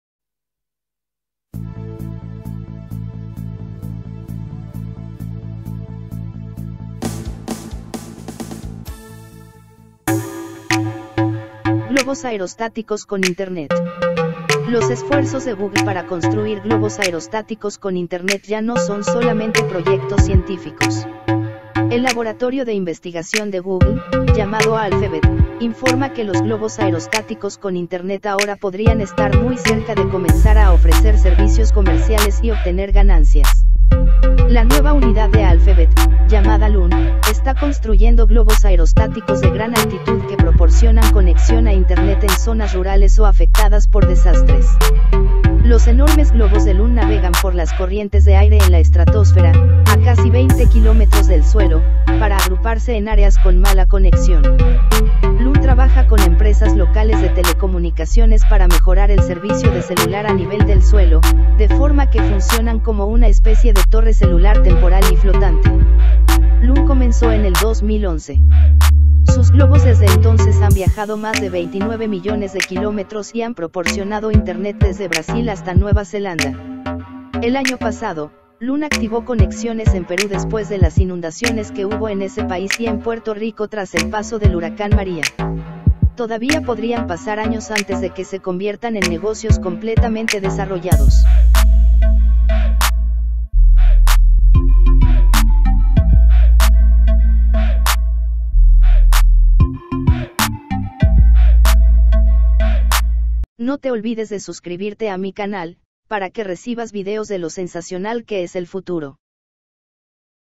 Globos con conexión a internet
Los esfuerzos de Google para construir globos aerostáticos con internet ya no son solamente proyectos científicos.
el laboratorio de investigación de Google, llamado Alphabet, informa que los globos aerostáticos con internet ahora podrían estar muy cerca de comenzar a ofrecer servicios comerciales y obtener ganancias.
La  nueva unidad de Alphabet, llamada Loon, está construyendo globos aerostáticos de gran altitud que proporcionan conexión a internet en zonas rurales o afectadas por desastres. Los enormes globos de Loon navegan por las corrientes de aire en la estratósfera, a casi 20 kilómetros del suelo, para agruparse en áreas con mala conexión. Loon trabaja con empresas locales de telecomunicaciones para mejorar el servicio de celular a nivel del suelo, de forma que funcionan como una especie de torre celular temporal y flotante.